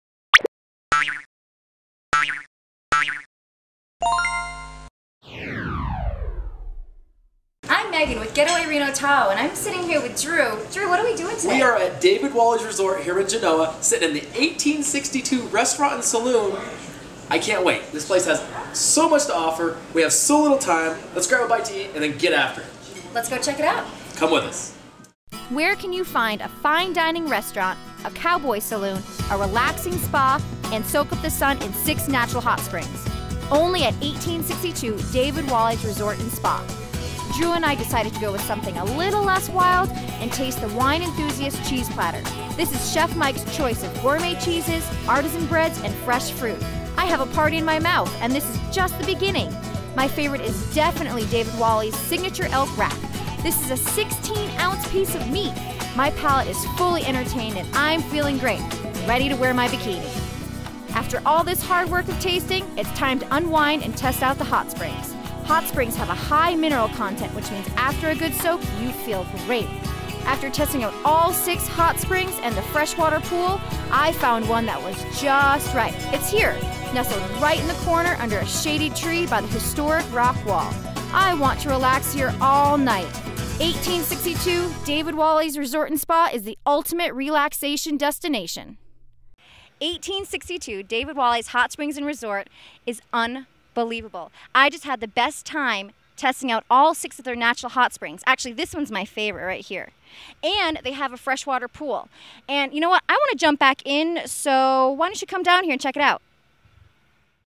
1862 David Walley's Resort
Getaway Reno Tahoe Visits 1862 David Walley's Resort, one of Reno Tahoe's premier destinations in the heart of the Carson Valley.